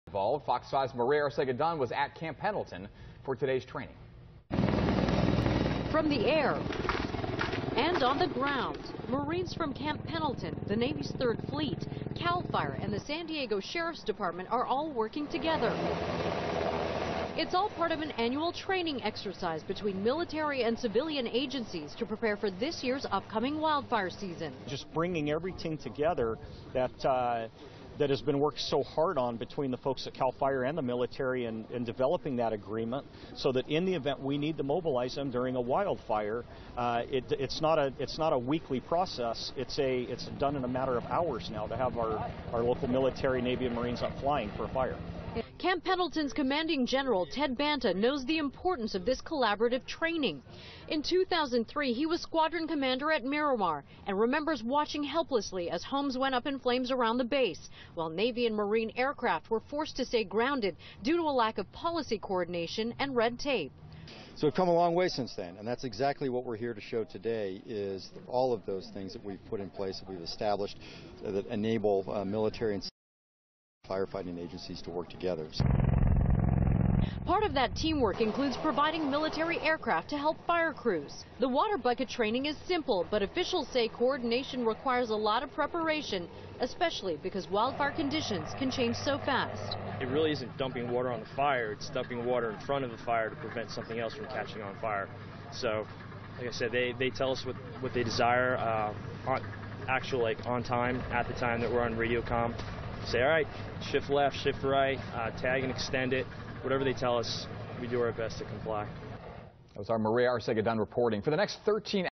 Wildland Fire Fighting Fox5
A wildland firefighting exercise involving aviation and ground units from Camp Pendleton, 3rd Marine Aircraft Wing, Navy Region Southwest, Third Fleet, CALFIRE, and the San Diego Sheriff's Department will took place May 5, 2016.This exercise provided ready, trained and certified military and civilian resources to combat wildland fires in the region and culminates with a cooperative effort to extinguish a simulated wildland fire. Marine Corps, Navy, and civilian helicopters will be supported Camp Pendleton and CALFIRE ground teams in establishing a simulated fire line and conducting water bucket operations.